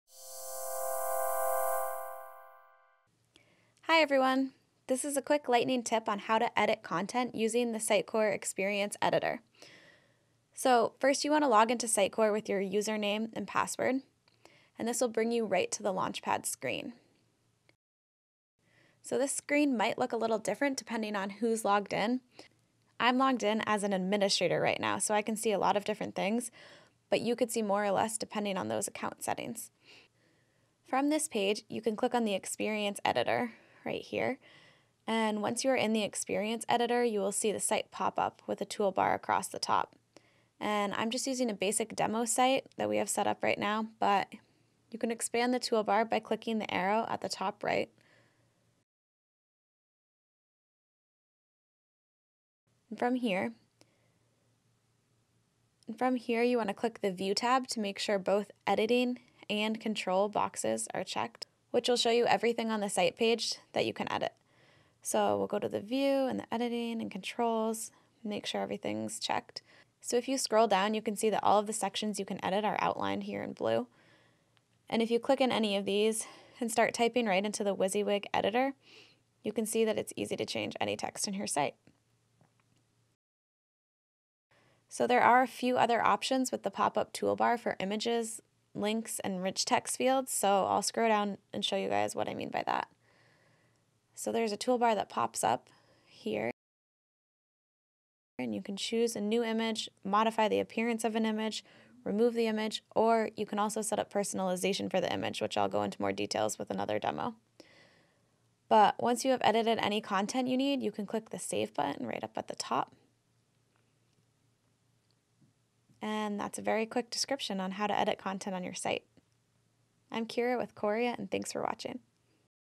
Using Sitecore Experience Editor to Edit Content
How to use Sitecore Experience Editor to edit content.